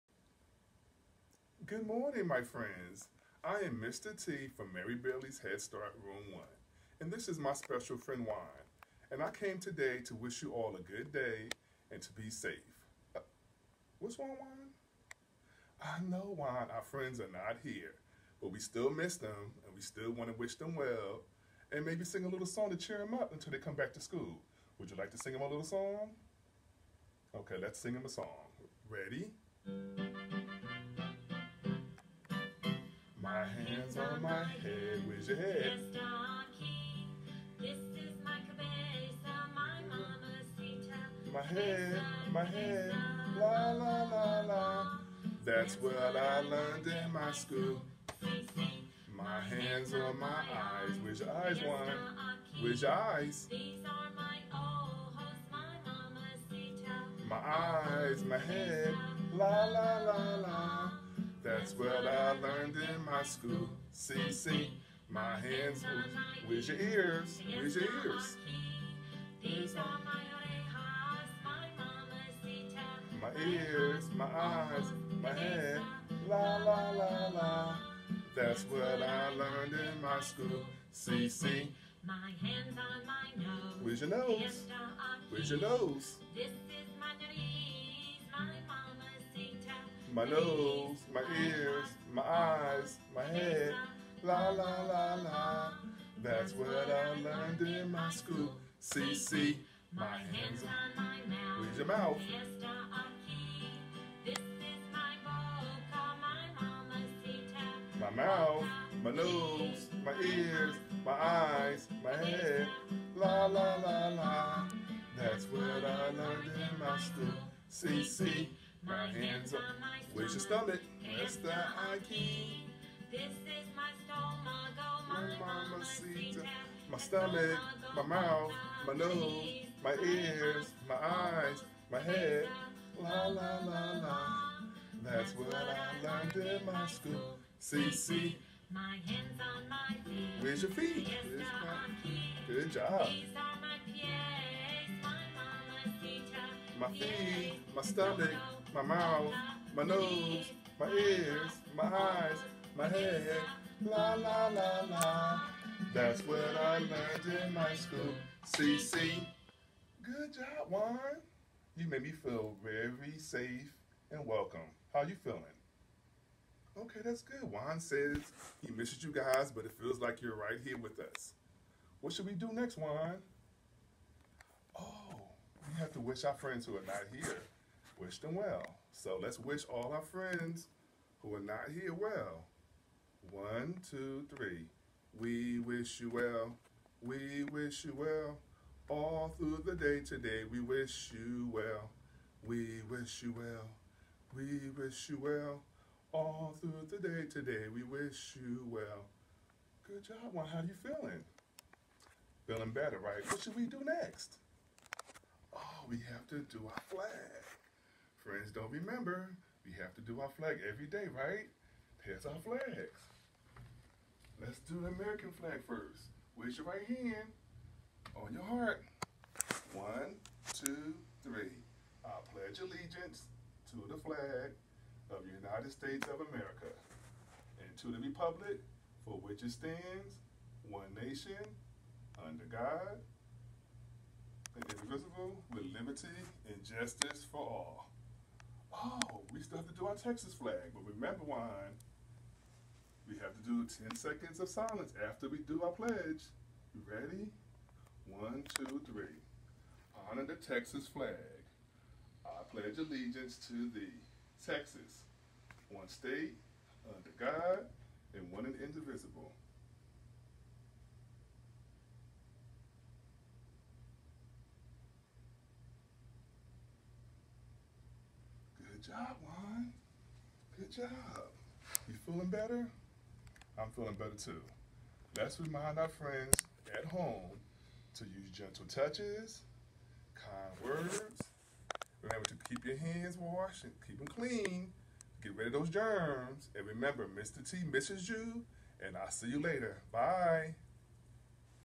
Mary Bailey -Mr. T's Song and Pledge
Mary Bailey Head Start's Mr. T shares a song and does the pledge of allegiance.